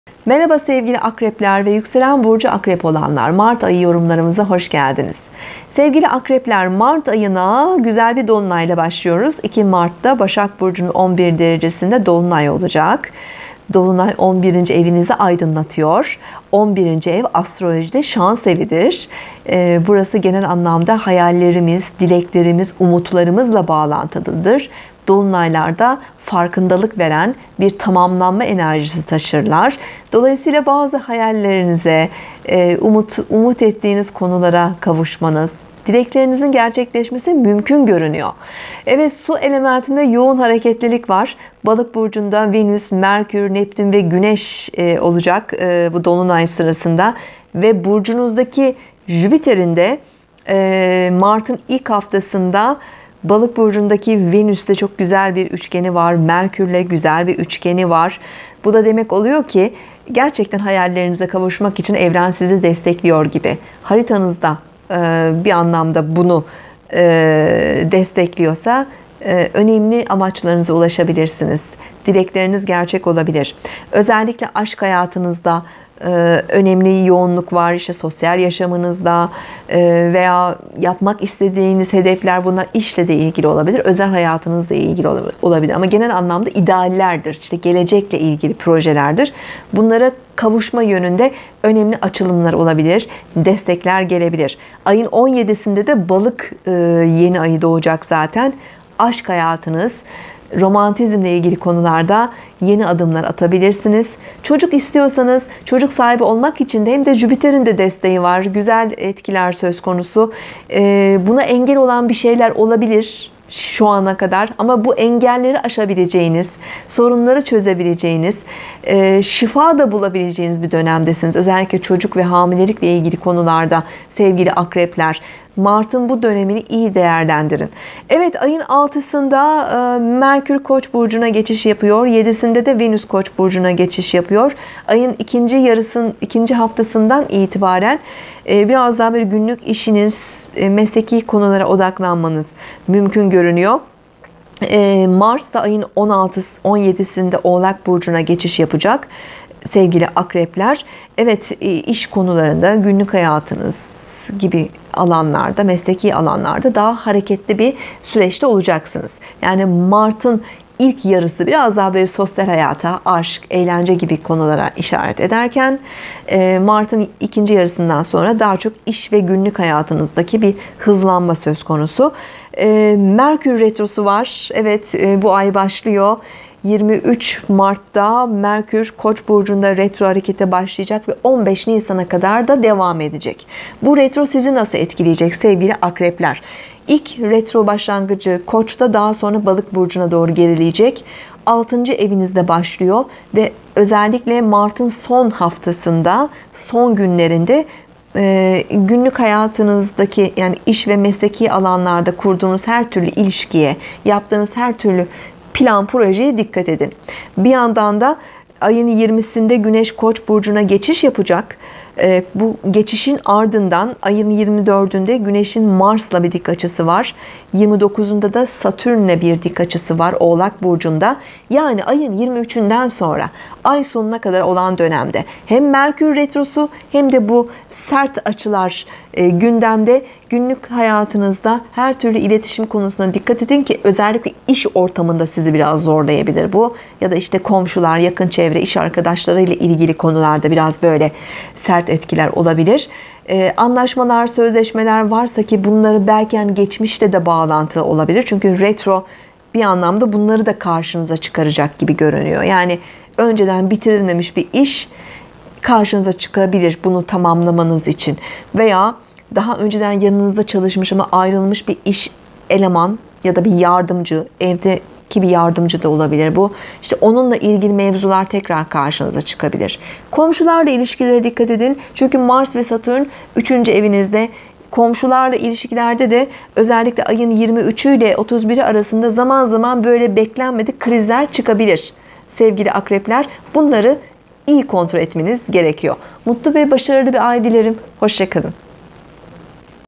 ♏ Akrep Burcu ♏ Mart 2018 Aylık Burç ve Astroloji Yorumu. Demet Baltacı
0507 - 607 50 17 - Kişisel doğum haritanız ve danışmanlık için  arayabilirsiniz.

2018 Günlük burç yorumları, haftalık ve aylık burç yorumları 2018 yılı videoları ve genel astroloji değerlendirmeleri.